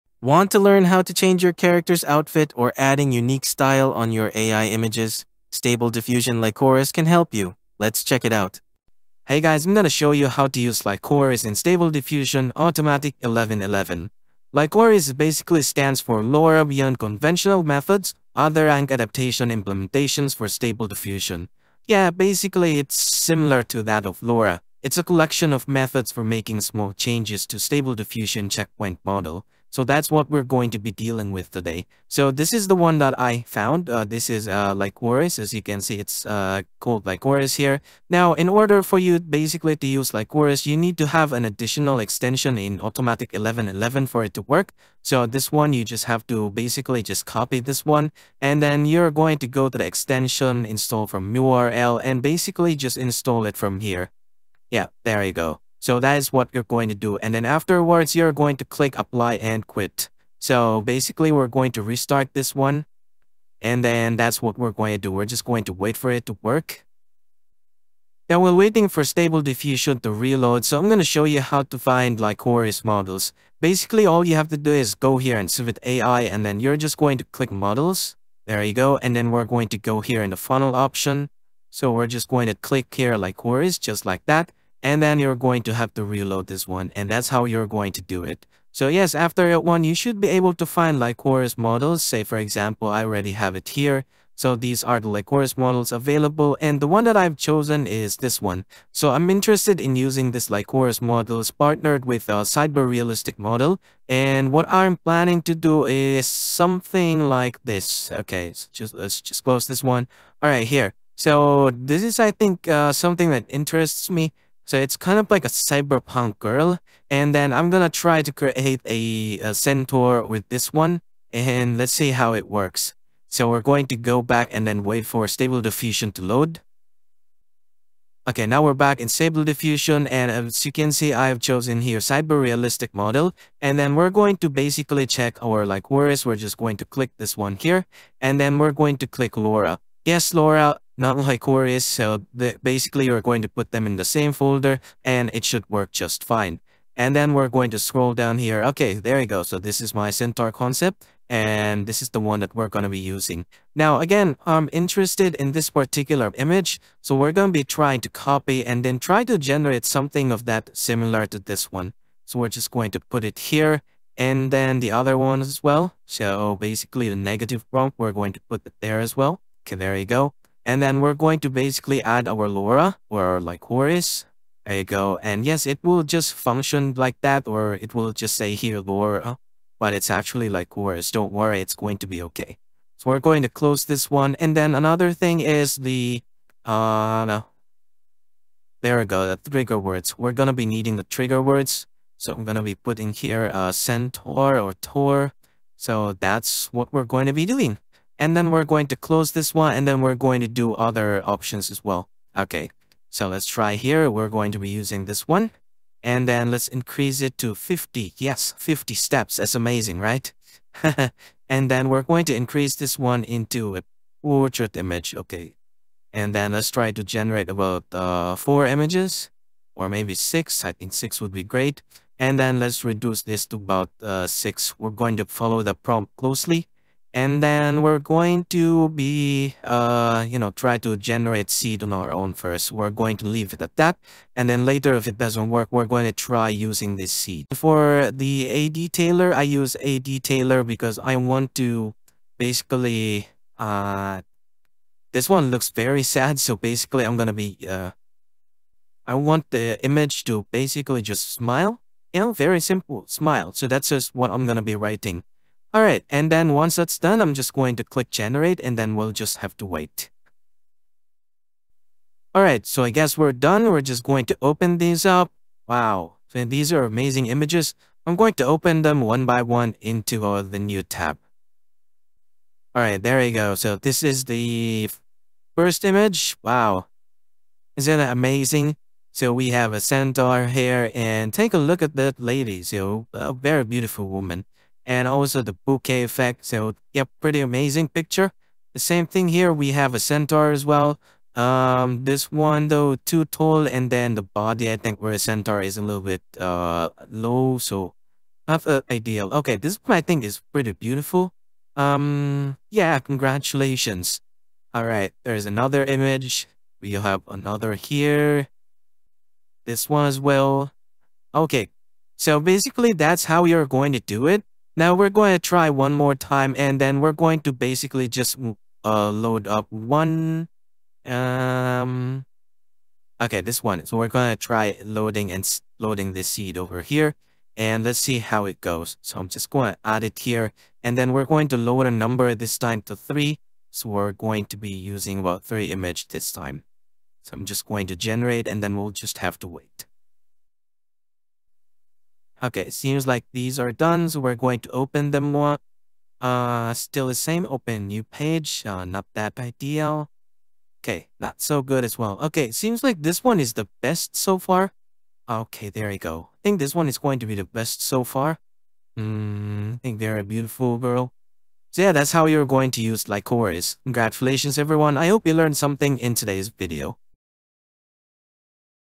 How To Use Stable Diffusion Models With Lycoris To Add Unique Style On AI Images (Tutorial Guide)
In this tutorial, discover how to elevate your AI-generated images by changing character outfits and infusing them with distinct styles using Stable Diffusion Lycoris. Join us as we delve into the world of Lycoris, a cutting-edge extension for Stable Diffusion Automatic 1111. Licorice, an acronym for "Lora beyond conventional methods other ank adaptation implementations for stable diffusion," is a revolutionary collection of techniques that enable subtle yet powerful alterations to Stable Diffusion checkpoint models. Drawing parallels with Lora's capabilities, Lycoris offers a spectrum of methods to enhance your AI-generated visuals.

To begin, we'll guide you through the setup process. You'll learn how to add the essential Lycoris extension to your Automatic 1111 interface, ensuring seamless integration for creating stunning image transformations. We'll explore how to access Lycoris models by navigating through CivitAI and the model funnel. Then, armed with the chosen Lycoris model, we'll dive into applying it to your AI images, accompanied by engaging visuals that capture the essence of each step.

As we proceed, you'll witness the transformative power of Lycoris in action. We'll demonstrate the process of generating captivating images with distinct styles, from cyberpunk aesthetics to centaur concepts. Dive into the experimentation phase, where we adjust parameters like image steps, portrait settings, and trigger words to customize results. With a hands-on approach, you'll witness the potential of Licorice to bring your creative visions to life. Join us on this journey of AI-driven style transformation and artistic exploration.